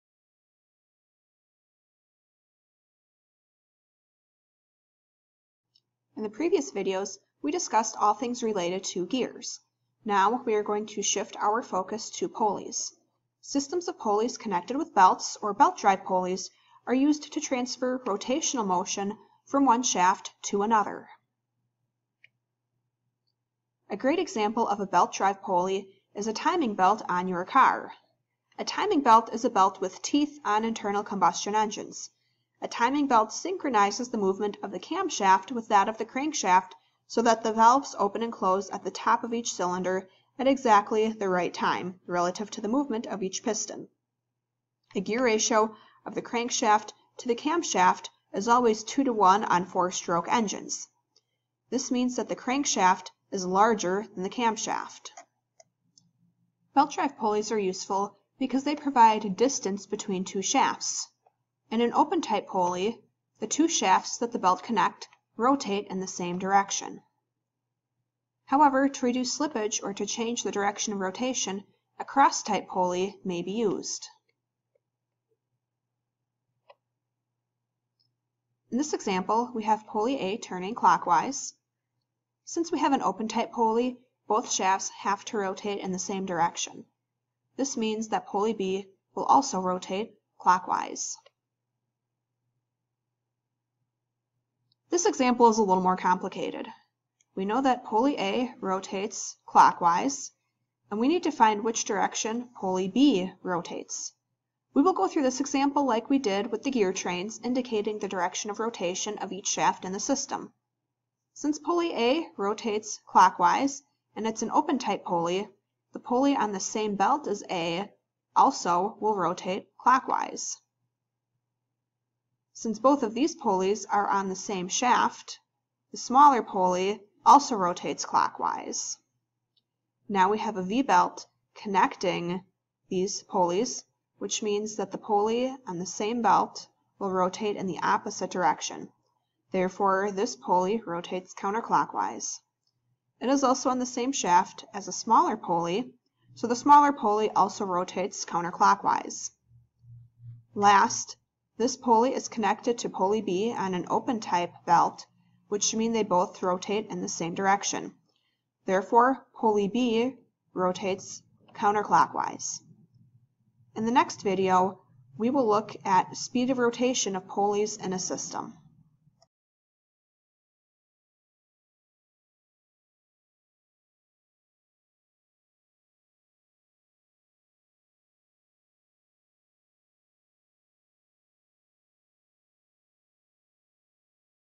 Belt Drive Pulleys – Science of Mechanics
Learn about belt drive pulleys and how to find the direction of rotation.

Chapter 4 Section 2 Part 6 Lecture video for Science of Mechanics
This lecture video for Science of Mechanics is presented by Southwest Wisconsin Technical College mathematics/science instructor Amanda Vissers.